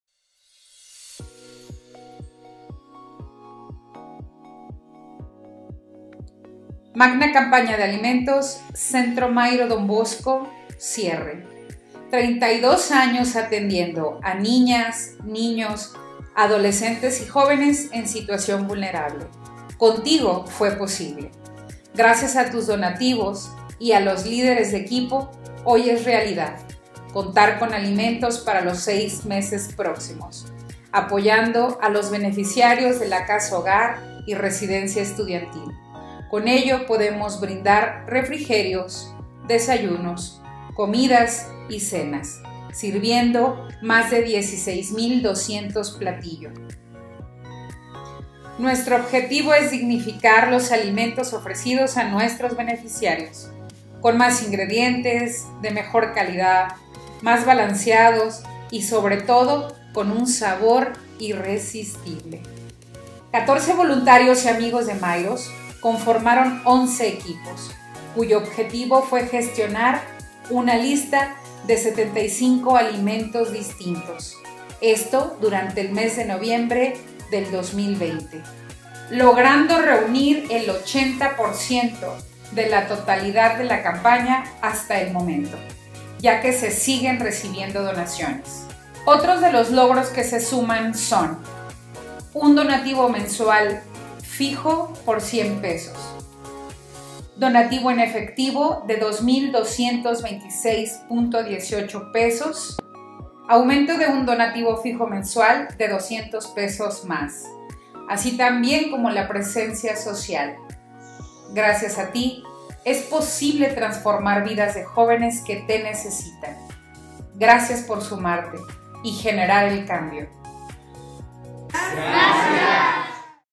INFORME MAGNA CAMPAÑA DE ALIMENTOS CENTRO MAIRO DON BOSCO NOVIEMBRE 2020
Durante el mes de Noviembre se llevó a cabo la Magno Campaña de Alimentos 2020, cuyo objetivo es fortalecer los alimentos que se sirven en las Casas Hogar, apoyando a jóvenes en situación vulnerable.

A través de este video agradecemos a las personas por su donativo, gracias.

Donativos BBVA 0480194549, Guadalajara Jalisco, México.